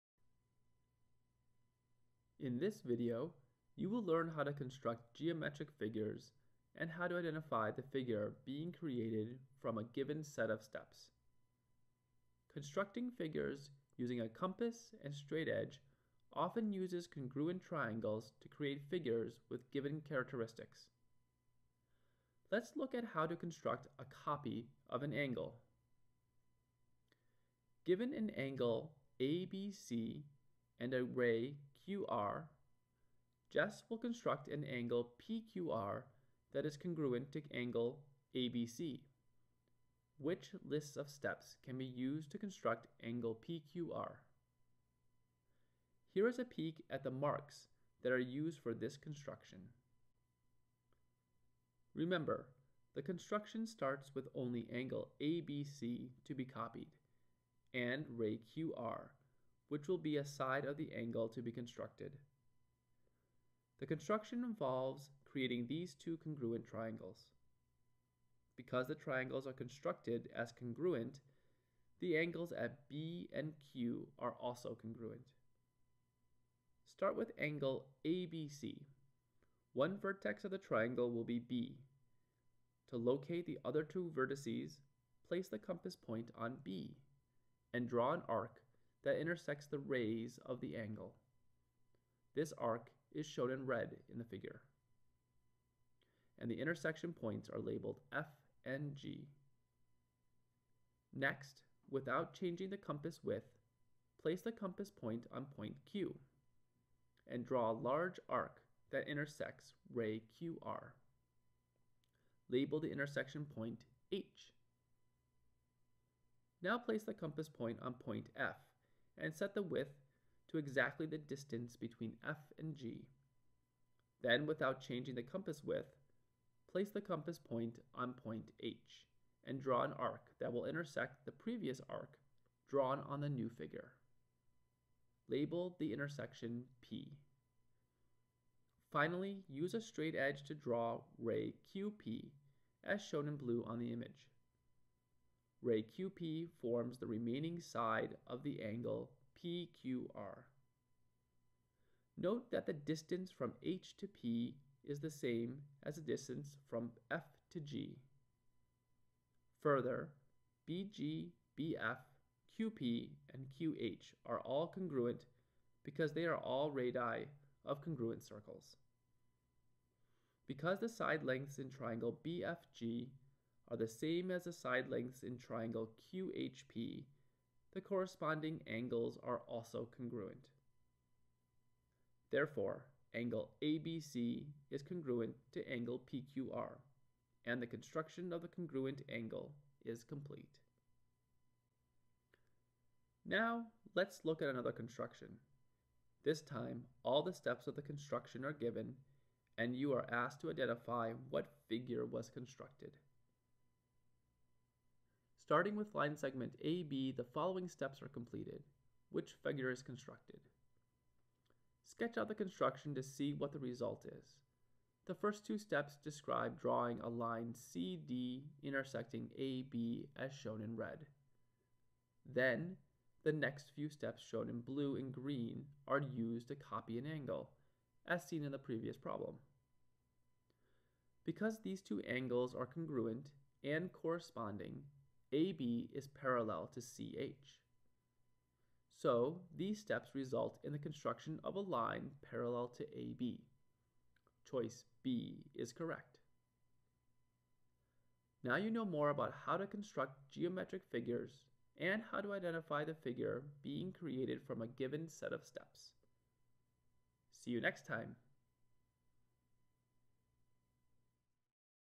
Constructing Geometric Figures: G-CO.12
This lesson explains how to construct geometric figures and how to identify the figure being created from a given set of steps.